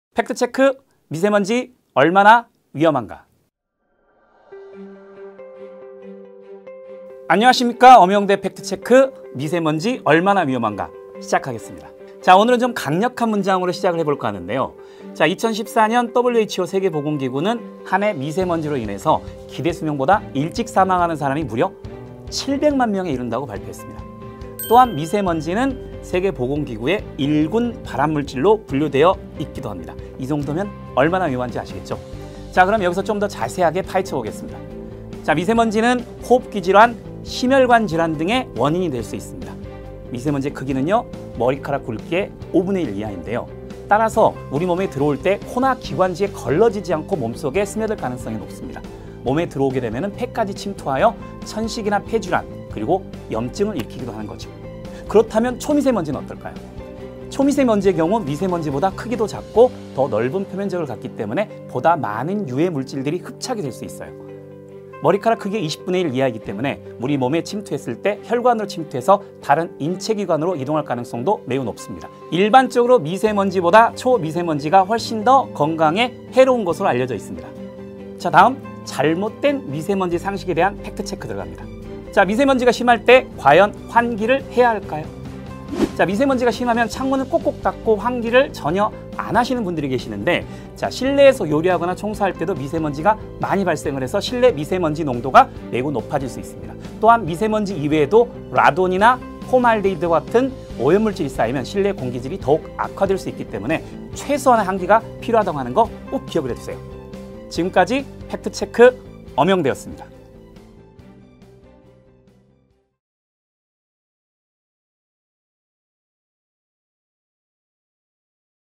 [메가스터디] 지구과학 엄영대 쌤 - 미세강의 제 3강! 엄영대의 팩트체크! 미세먼지 얼마나 위험한가?
미세강의 제 3강! 엄영대의 팩트체크! 미세먼지 얼마나 위험한가?

지구과학 전문가 엄영대 선생님과 함께하는 미세강의 시리즈
제3강 '엄영대의 팩트체크! 미세먼지
얼마나 위험한가?'
지금부터 함께 만나보시죠🎬

▶ 엄영대 선생님의 미세강의에 대해 미세하게 알고 싶다면?

▶엄영대 선생님의 강좌에 대해 더 알고싶다면?